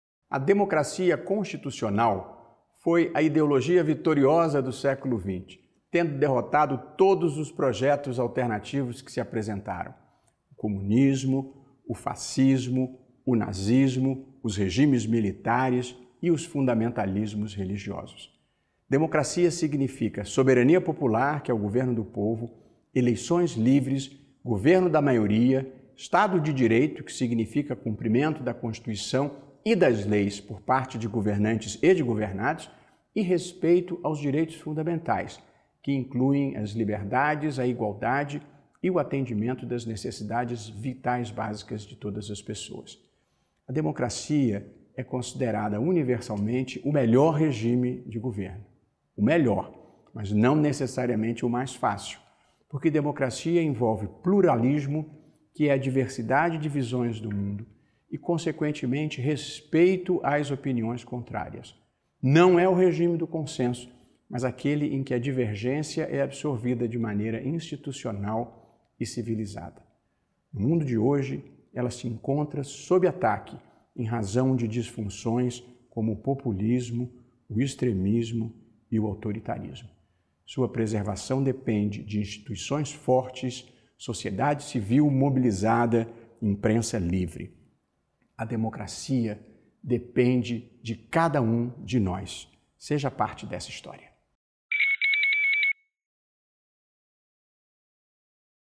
Mensagem de Luís Roberto Barroso sobre o Dia da Democracia (2021)
No Dia Internacional da Democracia, 15 de setembro, o ministro do Supremo Tribunal Federal (STF) e presidente do Tribunal Superior Eleitoral (TSE), Luís Roberto Barroso, fala sobre características e fenômenos do regime político do povo, pelo povo e para o povo.